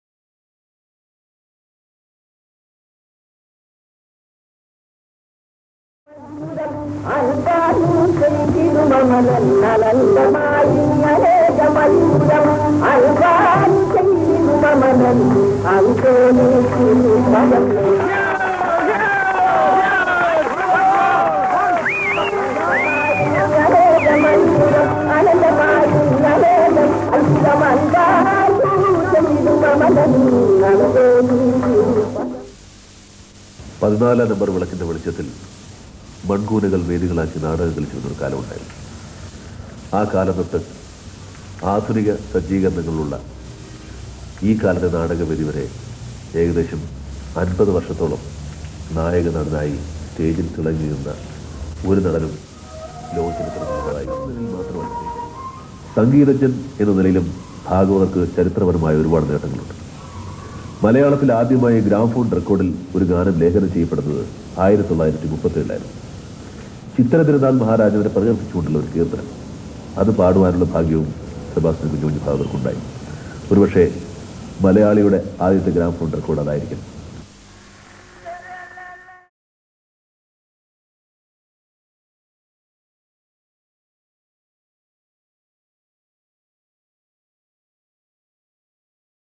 Innalayude Superstar - Kunju Kunju Bhagavathar
Excerpts from a 1980s Documentary film on my grand father \
Sebastian Kunju Kunju Bhagavathar
Superstar Mammooty remembers Bhagavathar